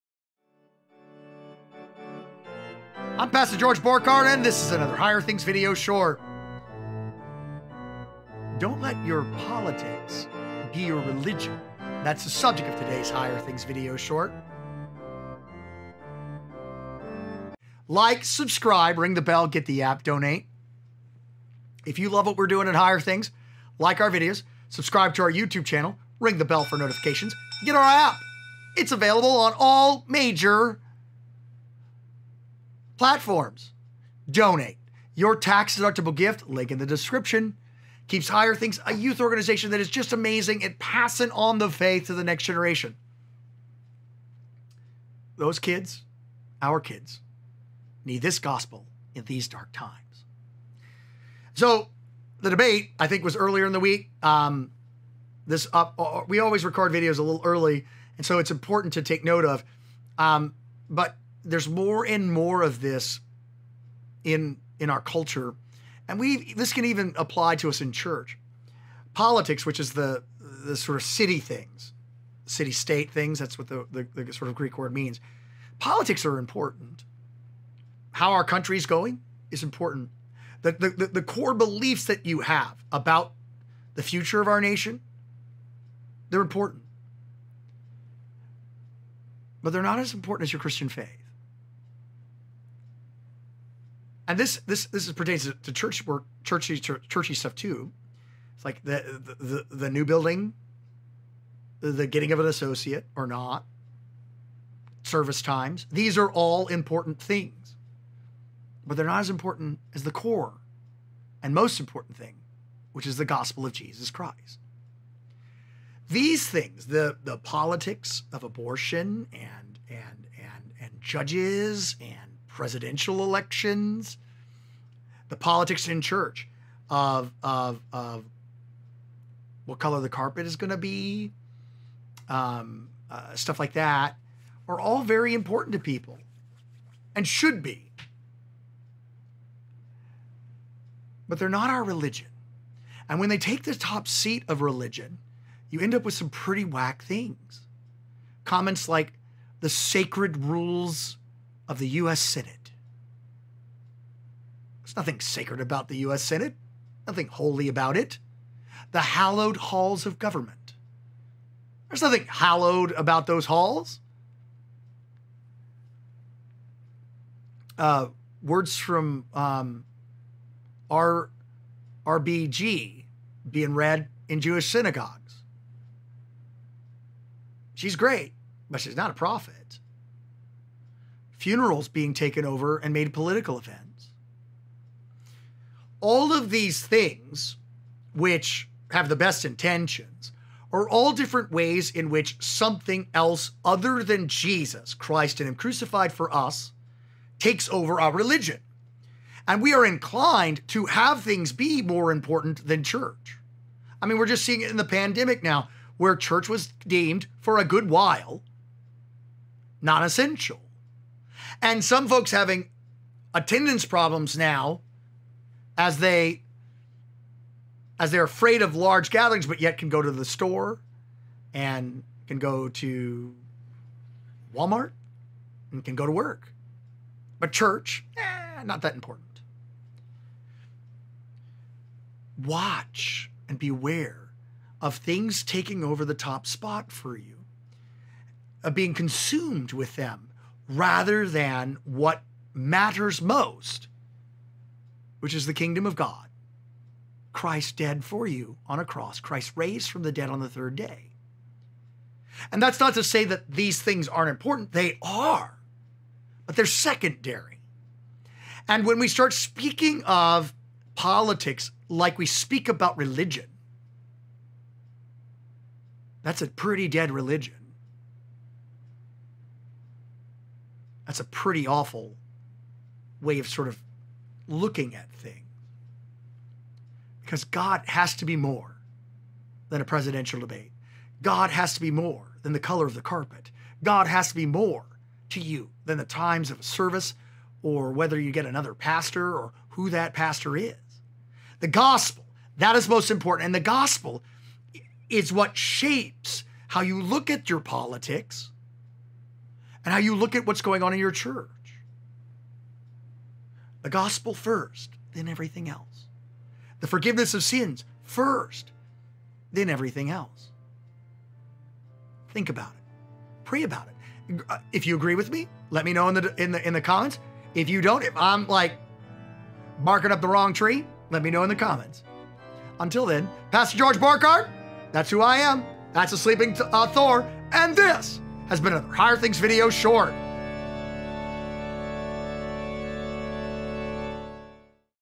1001 The Danger of Politics Taking Over Our Religion - A Higher Things® Video Short
As we rumble towards the election we must remember to not let our politics become our religion. Is politics and the things we believe about politics important? Of course, but we need to watch and beware of the politics of our world consuming us. They’re important, but secondary to Christ crucified for you. The Gospel and the forgiveness of sins first, then everything else.